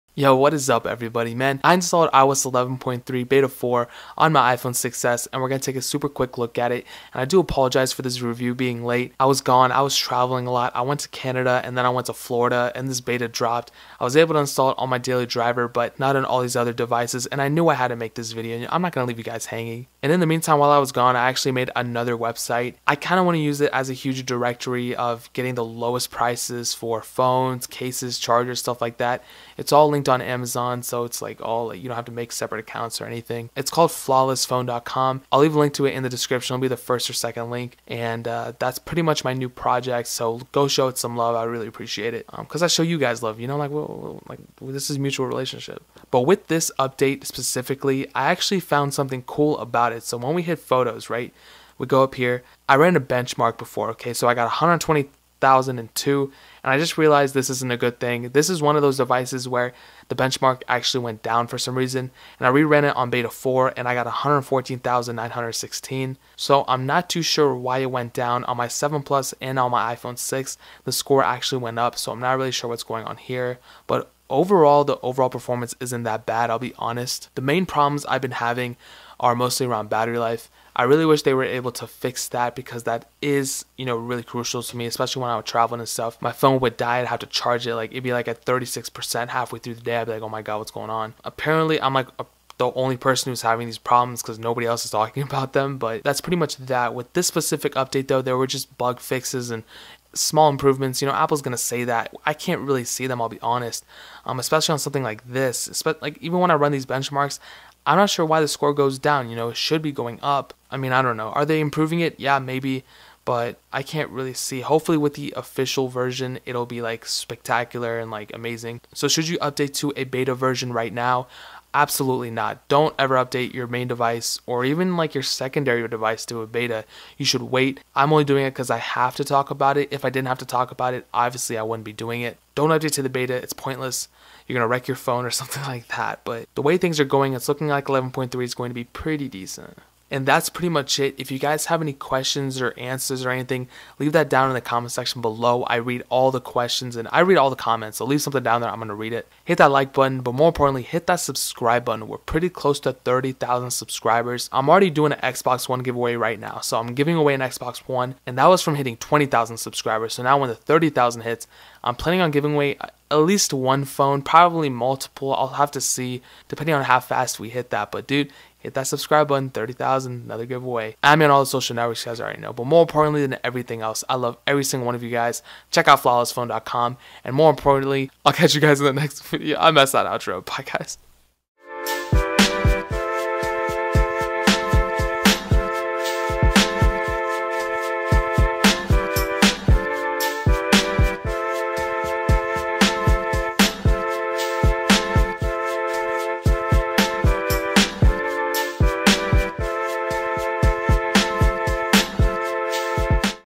iOS 11.3 BETA 4 On iPHONE 6S! (Review)
Phones I have laying around:
iPhone 6 Plus:
iPhone 6s Plus:
Google Pixel:
Nexus 5X:

The iOS 11.3 Beta 4 just got released! So let’s take a look at how this run on your iPhone!